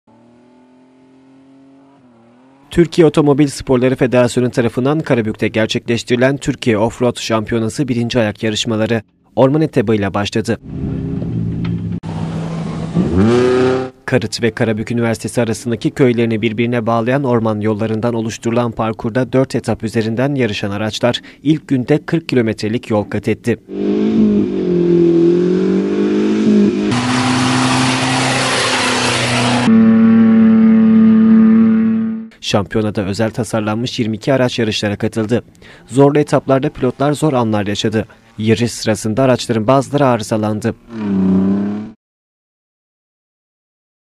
TÜRKİYE OFF-ROAD ŞAMPİYONASI
Karabük'te başlayan 1. ayak yarışlarının ilk gününde orman parkurunda 4 etap üzerinden yarışan özel tasarlanmış araçlar, 40 kilometrelik yol kat etti.